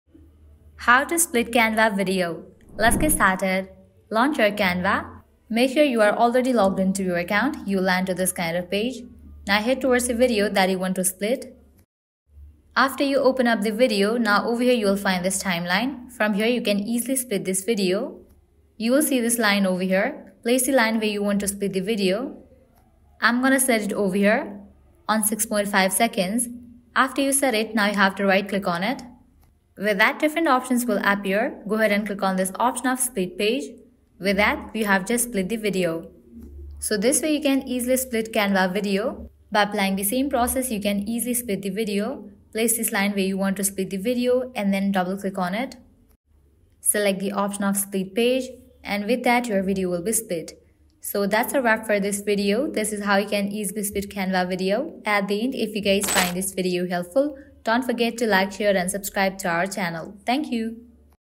How to Split Canva Video (UPDATED)
Are you ready to elevate your video editing skills with Canva in 2024? Look no further! This comprehensive guide is your ultimate resource for mastering the split screen effect in Canva videos. Whether you're a content creator, marketer, or enthusiast, this tutorial will equip you with the knowledge to create captivating videos that stand out.

In this detailed tutorial, we'll cover:

Step-by-step instructions on how to create a split screen effect using Canva's intuitive interface.
Insider tips for achieving seamless transitions and visual harmony between split screen elements.
Navigating Canva's vast library of stock footage, images, and graphics to enhance your split screen compositions.
Strategies for adding text, overlays, and animations to enhance the storytelling aspect of your split screen videos.
Updates and changes to Canva's video editing features in 2024, ensuring you stay ahead of the curve.
Get ready to unleash your creativity and produce stunning split screen videos that leave a lasting impression. If you've ever wondered, "How can I master the split screen effect in Canva?" - you're in the right place.

Let's make 2024 a year of visually engaging content and unparalleled creativity!